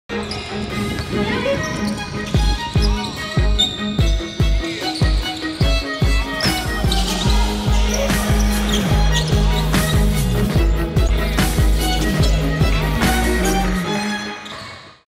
Basquete Transforma SC teaser 3
Teaser do projeto Basquete Transforma SC, uma parceria com a Federação Catarinense de Basket-ball que vai mostrar todo o potencial transformador do esporte em Santa Catarina.